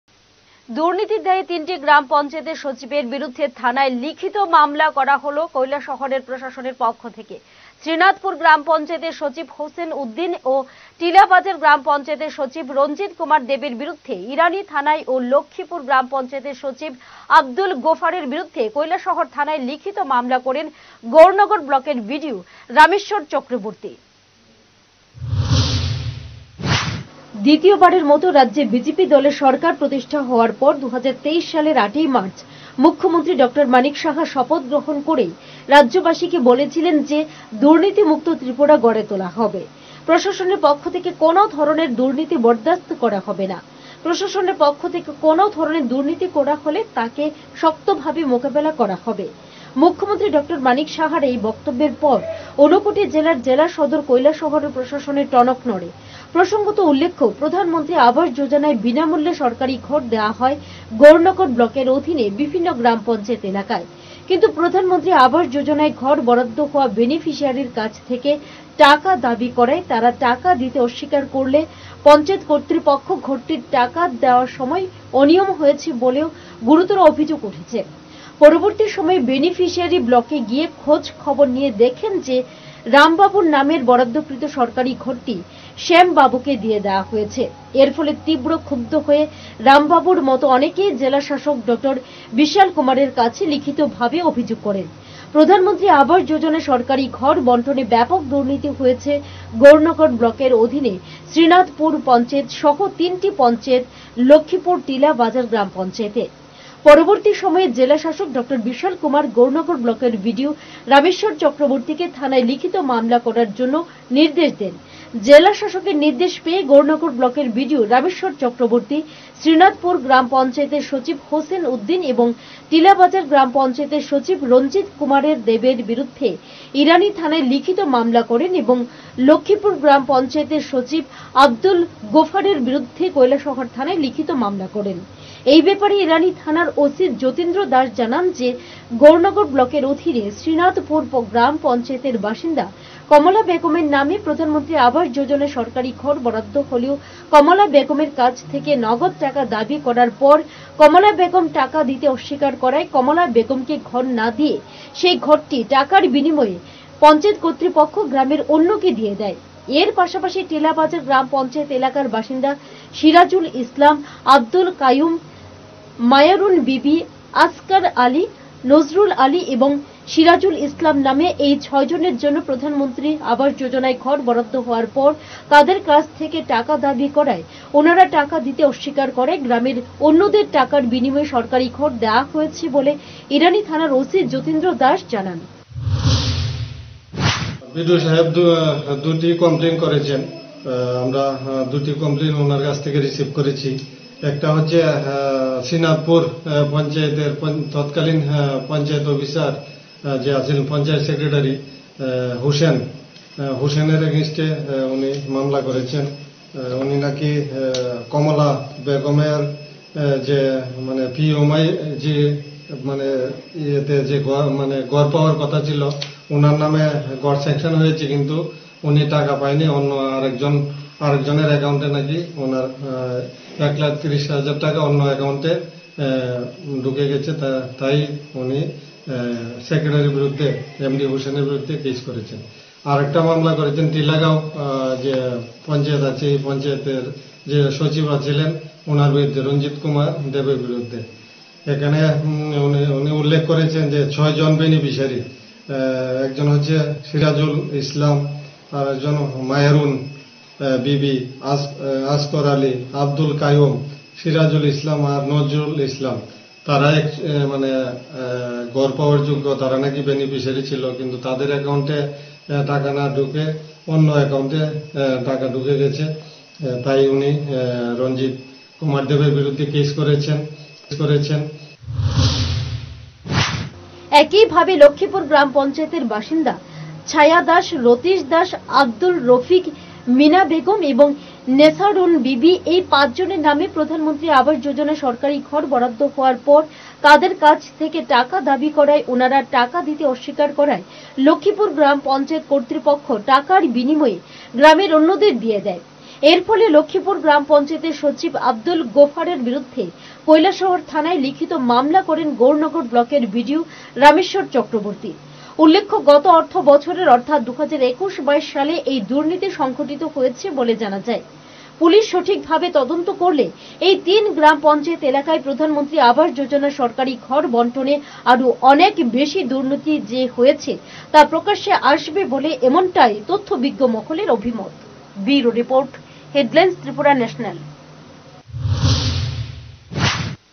তিনটি গ্রাম পঞ্চায়েতের সচিবের বিরুদ্ধে থানায় লিখিত মামলা করা হল কৈলাসহরে প্রশাসনের পক্ষ থেকে।
দুর্নীতির দায়ে তিনটি গ্রাম পঞ্চায়েতের সচিবের বিরুদ্ধে থানায় লিখিত মামলা করা হল কৈলাসহরে প্রশাসনের পক্ষ থেকে। শ্রীনাথপুর গ্রাম পঞ্চায়েতের সচিব হোসেন উদ্দিন ও টিলাবাজার গ্রাম পঞ্চায়েতের সচিব রঞ্জিত কুমার দেবের বিরুদ্ধে ইরানি থানায় ও লক্ষীপুর গ্রাম পঞ্চায়েতের সচিব আব্দুল গোফফারের বিরুদ্ধে কৈলাসহর থানায় লিখিত মামলা করেন গৌরনগর ব্লকের বিডিও রামেশ্বর চক্রবর্তী।